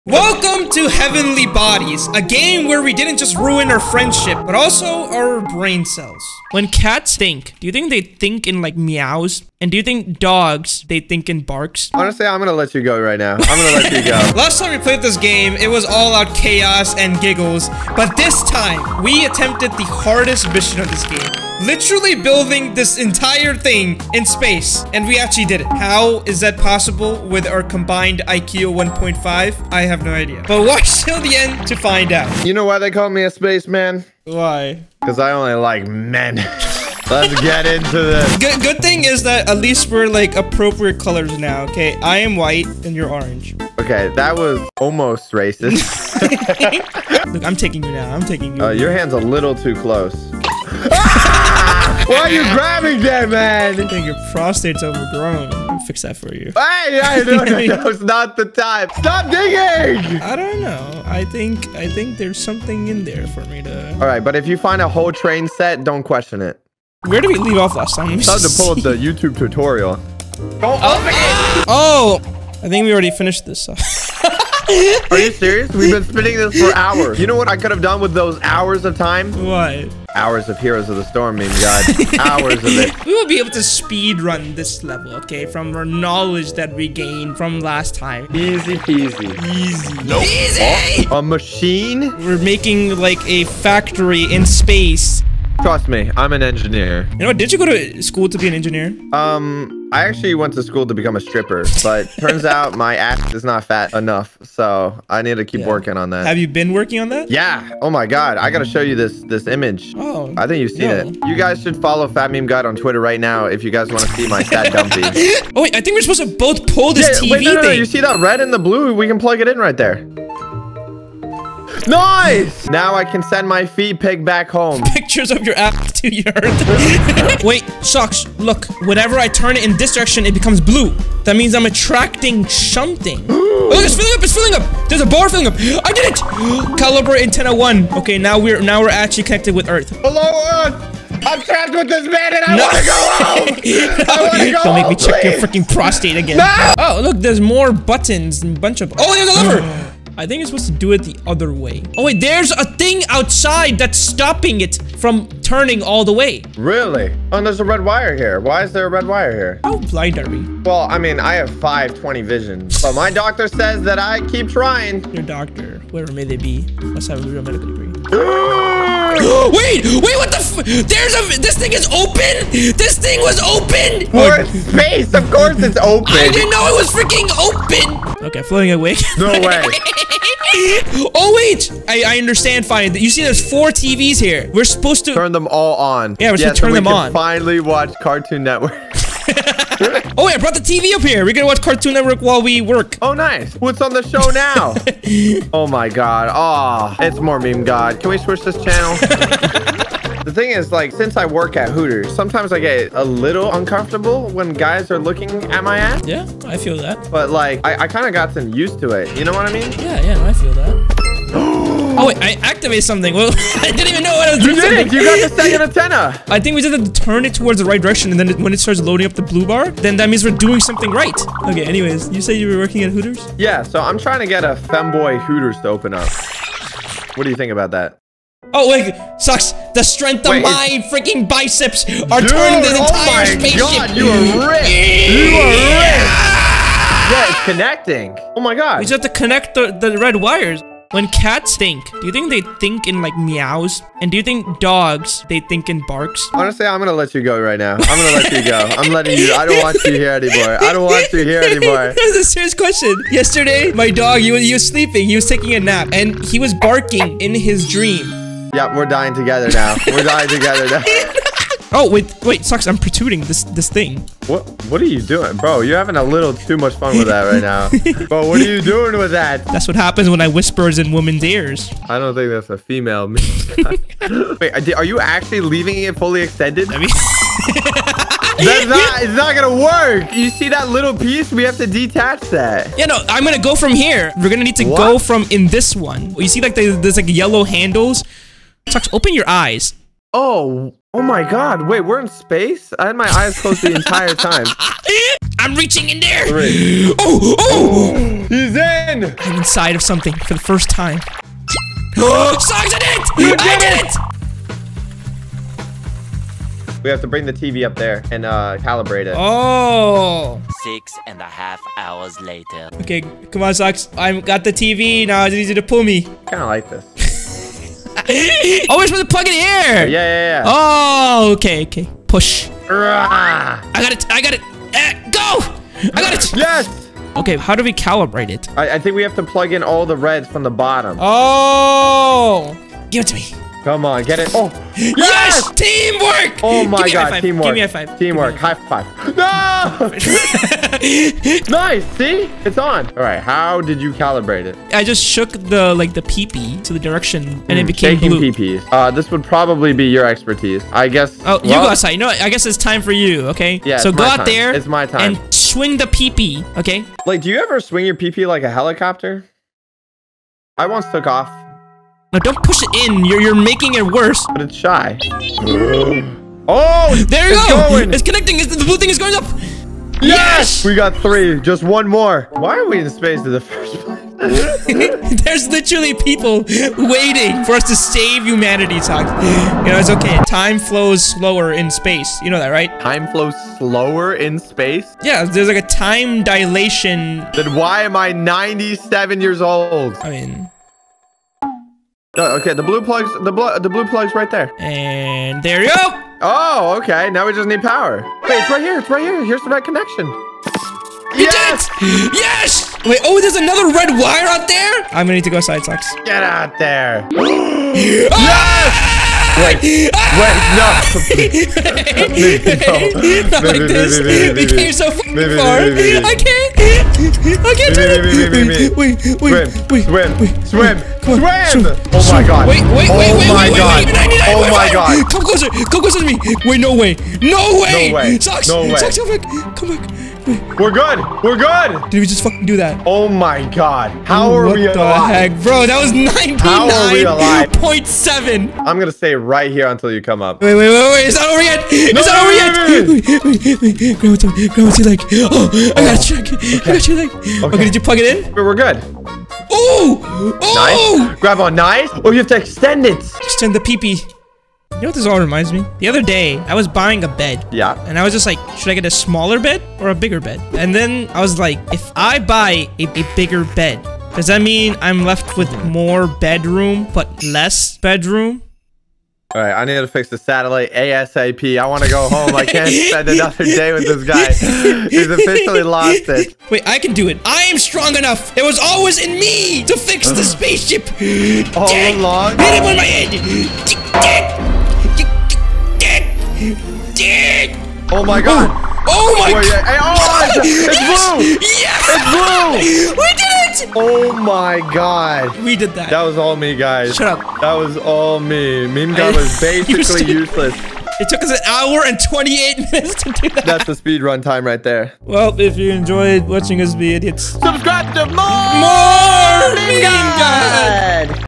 This Ruined Our Friendship
i took @Socksfor1  to space again LOL

Friends: @FatMemeGod

Also leave me some recommendations in the comments for what game to play, and I might make a vid on it :D

In this video, FatMemeGod (Morememegod) once again go to space to repair a broken space station to save humanity life on earth and get back home safely in their nasa rocket spacex space shuttle they communicate and get into funny situation and pranks